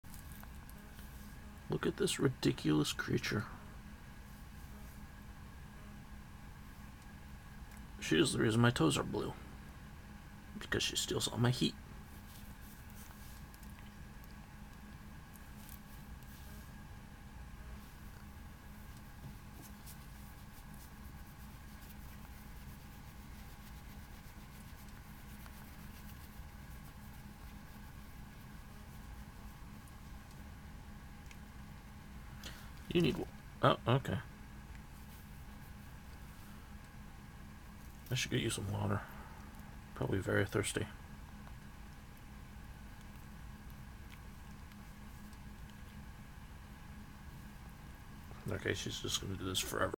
heat thief
cat steals all my heat